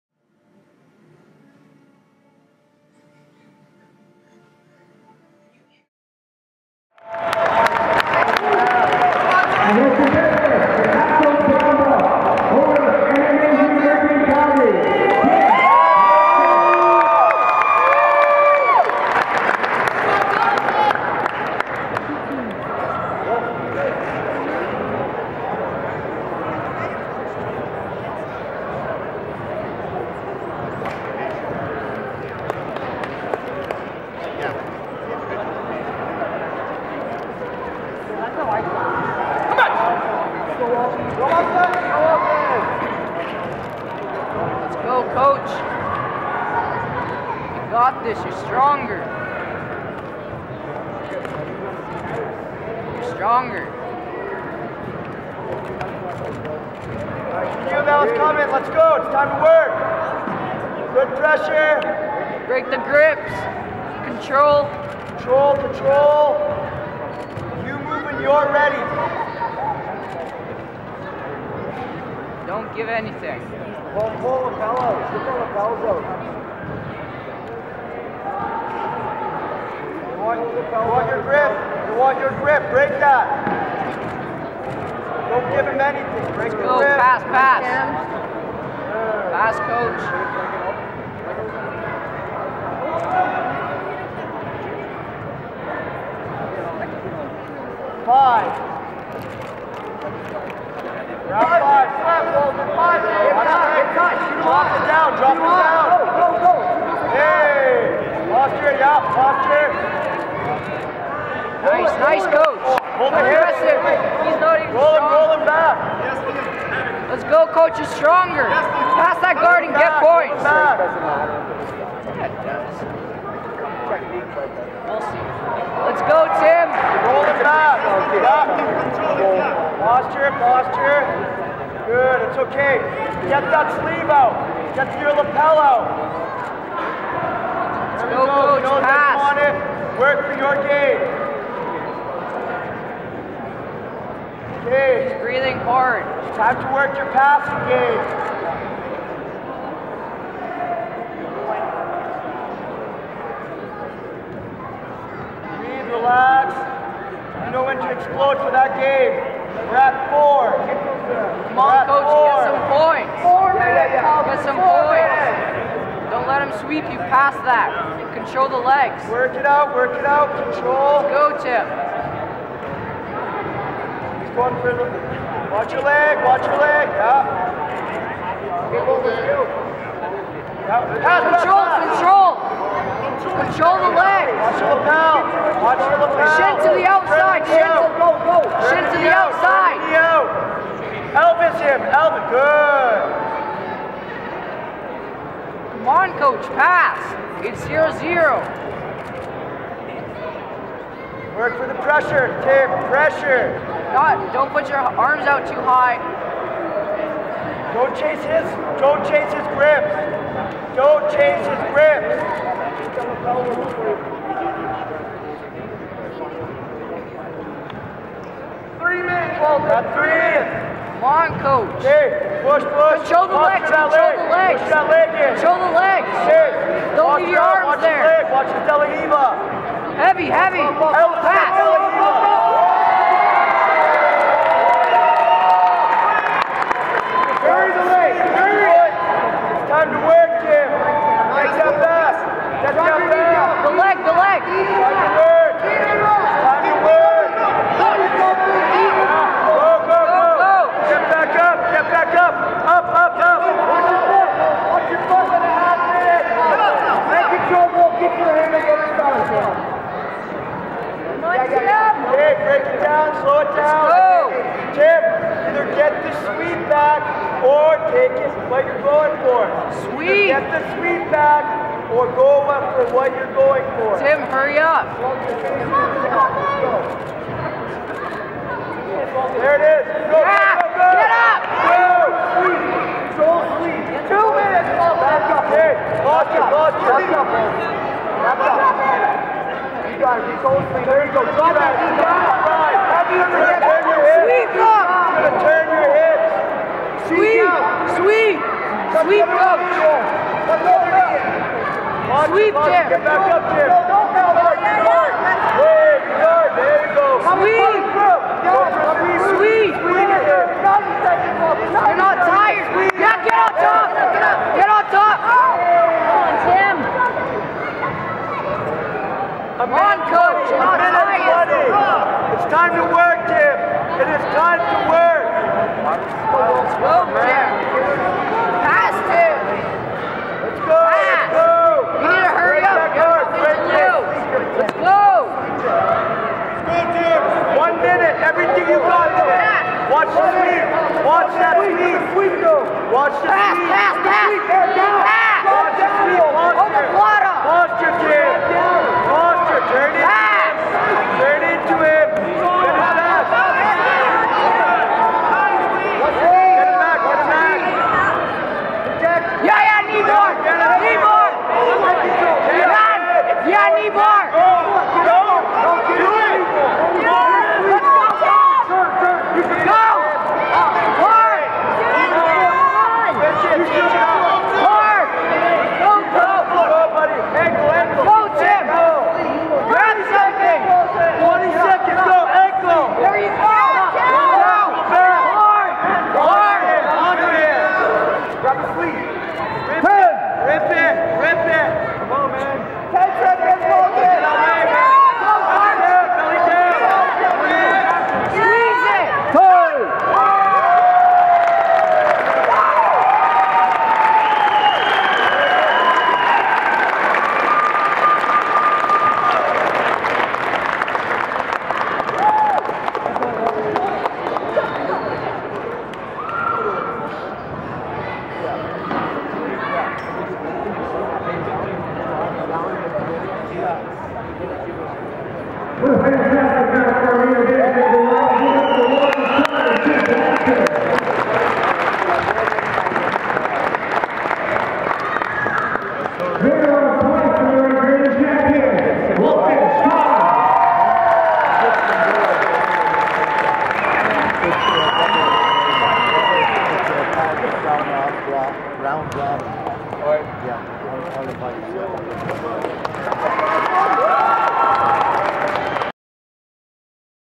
Walton Chung vs Tim Blanchard
This Main Event Brown Belt battle pitted two returning Champions against each other is an incredible display of high level Brazilian Jiu-Jitsu!